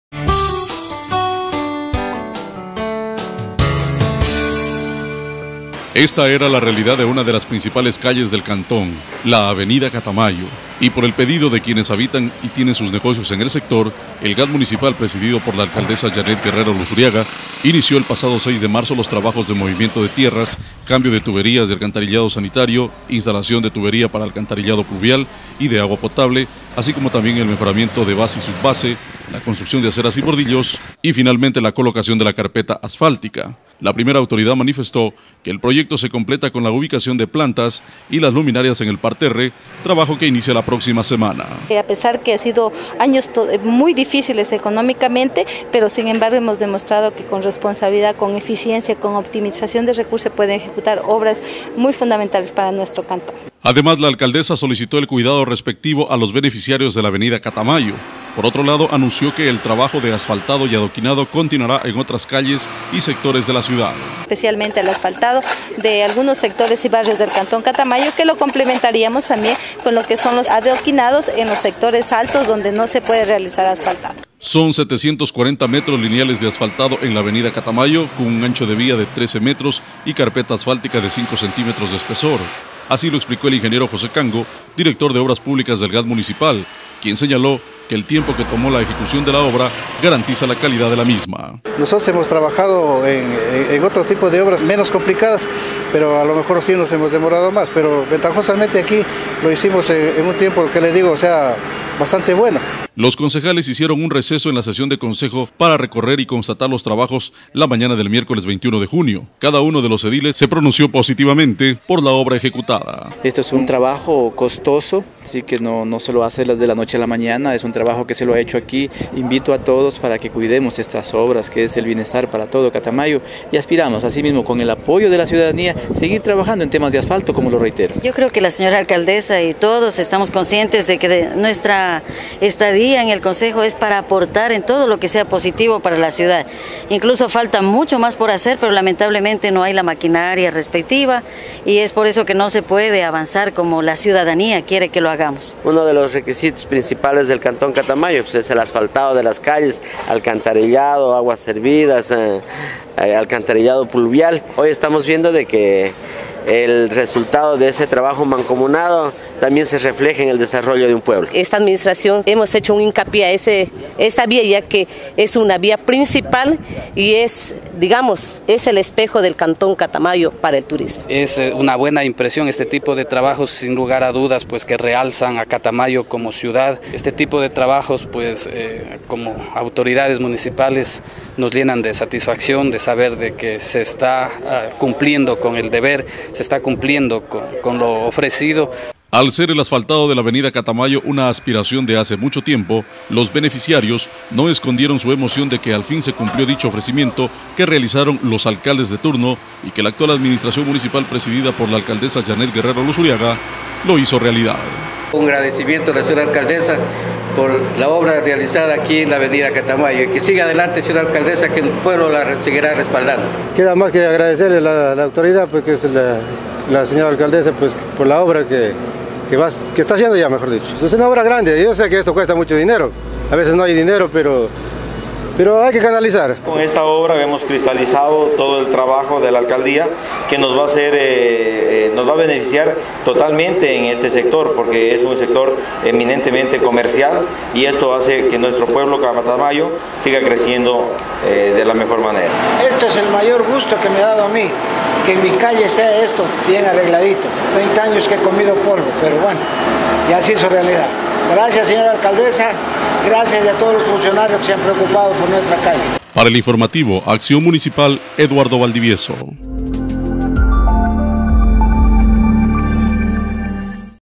Colocación de carpeta asfáltica en la Av. Catamayo
Esta era la realidad de una de las principales calles del cantón, la Avenida Catamayo, y por el pedido de quienes habitan y tienen sus negocios en el sector, el Gad municipal presidido por la Alcaldesa Janet Guerrero Luzuriaga, inició el pasado 6 de marzo los trabajos de movimiento de tierras, cambio de tubería de alcantarillado sanitario, instalación de tubería para alcantarillado pluvial y de agua potable, así como también el mejoramiento de la base y subbase, la construcción de aceras y bordillos, y finalmente con la colocación de carpeta asfáltica, la primera autoridad manifestó que el proyecto se completa con la ubicación de plantas y las luminarias en el parterre, trabajo que inicia la próxima semana.
Además la Alcaldesa solicitó el cuidado respectivo a los beneficiarios de la Avenida Catamayo, por otro lado, anunció que el trabajo de asfaltado y adoquinado, continuará en otras calles y sectores de la ciudad
Son 740 metros lineales de asfaltado en la Avenida Catamayo con un ancho de vía de 13 metros y carpeta asfáltica de 5 centímetros de espesor, así lo explicó el Ingeniero José Cango Director de Obras Públicas del Gad municipal, quien señaló que el tiempo que tomó la ejecución de la obra, garantiza la calidad de la misma.
Los Concejales hicieron un receso en la sesión de Concejo, para recorrer y constatar los trabajos la mañana del miércoles 21 de junio, cada uno de los ediles se pronunciaron positivamente por la obra ejecutada.
Al ser el asfaltado de la Avenida Catamayo una aspiración de hace mucho tiempo, los beneficiarios no escondieron su emoción, ya que al fin se cumplió dicho ofrecimiento que realizaron los alcaldes de turno, y que la actual administración municipal presidida por la Alcaldesa Janet Guerrero Luzuriaga, lo hizo realidad.